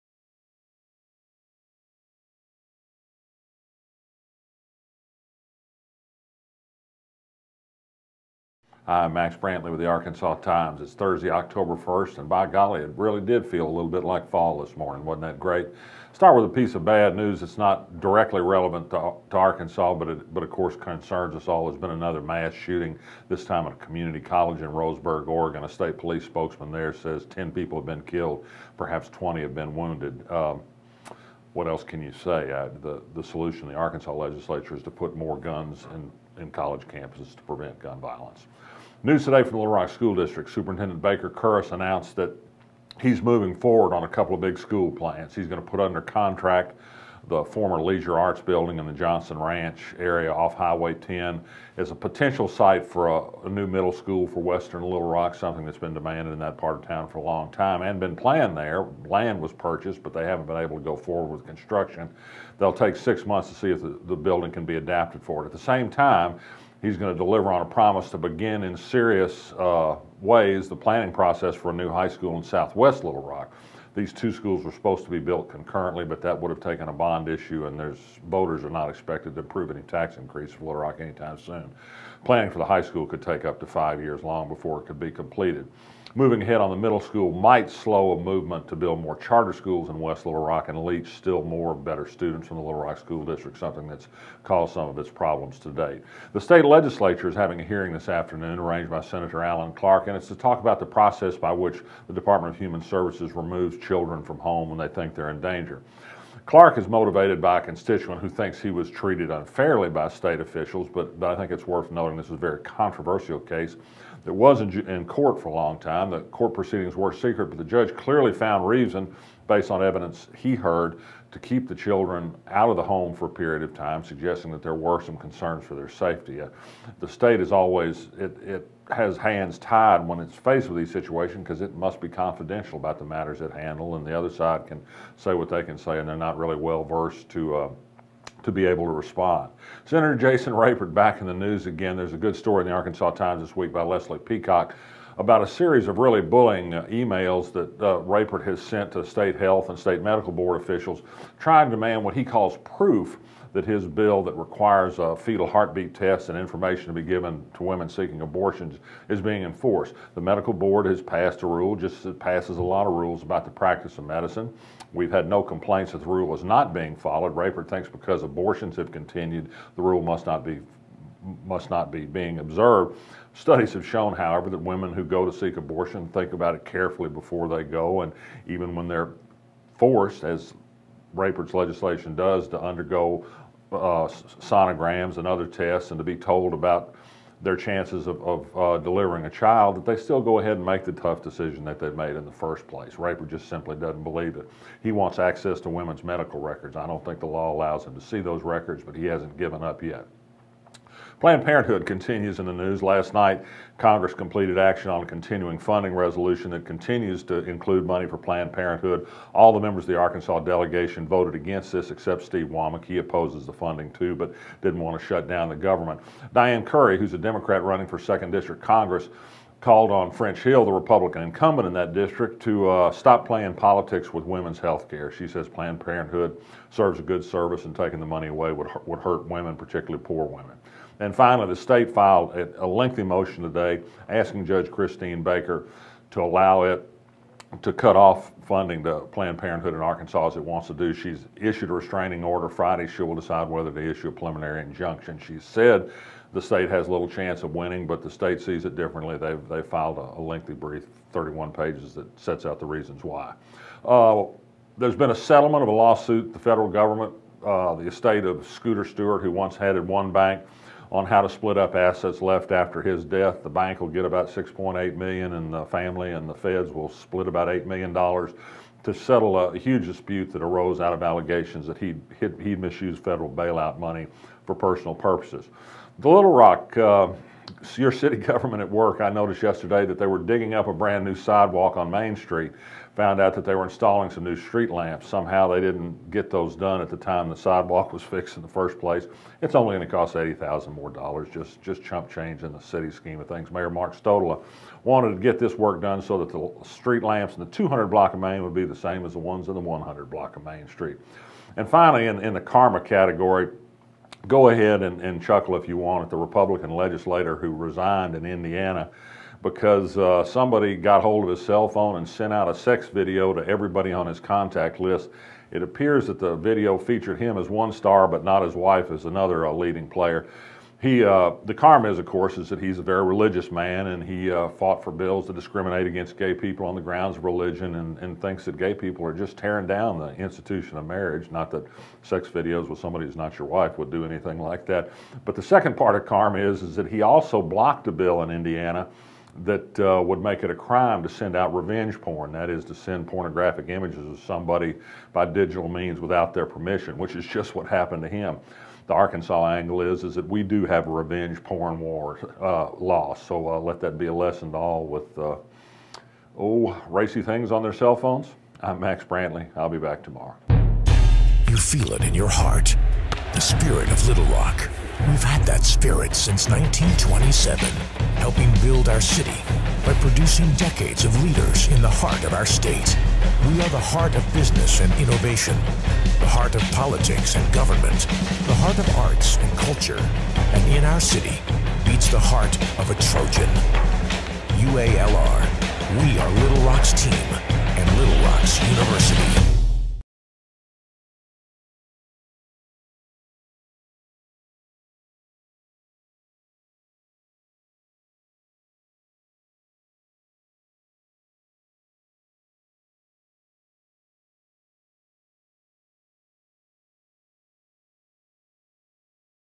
Today in Arkansas: Another mass shooting
News from Arkansas Thursday, Oct. 1. Another mass shooting in Oregon. Little Rock School District moves on new middle school. Jason Rapert goes after women's medical records. Planned Parenthood under attack. A do-over on Main Street. Revenge porn karma.